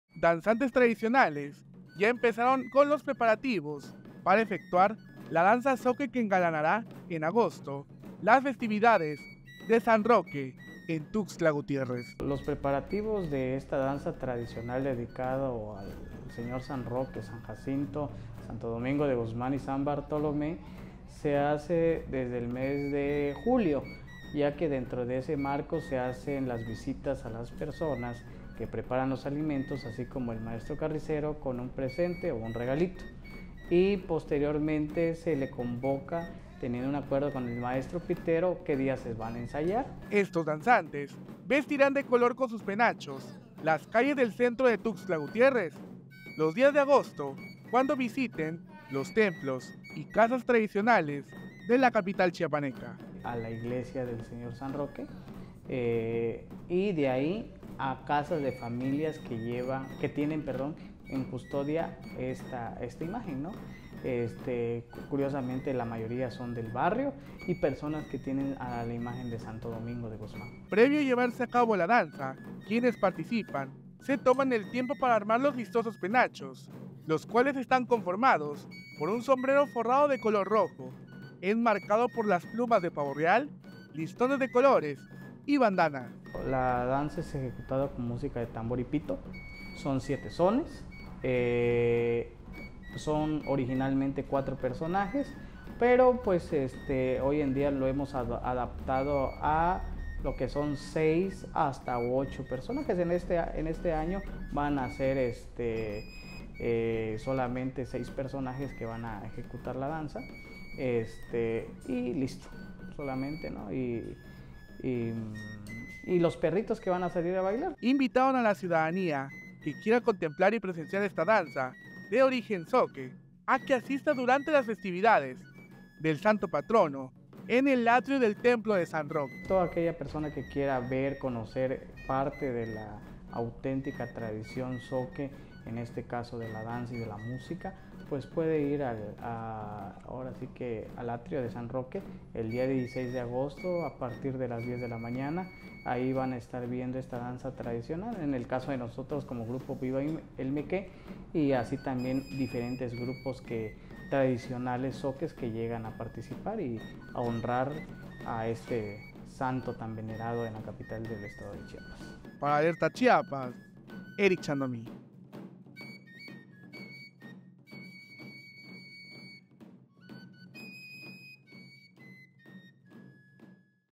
Preparativos de la Danza Zoque de San Roque en Tuxtla Gutiérrez
Los preparativos para la danza zoque de San Roque en Tuxtla Gutiérrez ya han comenzado. Los danzantes tradicionales, liderados por Victor Velázquez, están listos para engalanar las festividades de agosto con sus coloridos penachos y trajes. Esta danza, ejecutada con música de tambor y pito, es una tradición rica en historia y cultura zoque. Descubre más sobre cómo se preparan y cómo puedes ser parte de esta celebración en el atrio del templo de San Roque.